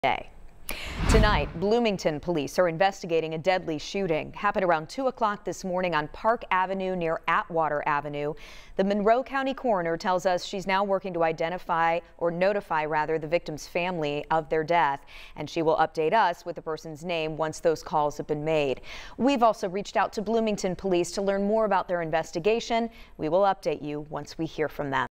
Bloomington police investigating deadly shooting
The Monroe County coroner tells 13News she's working to notify the victim's family of their death.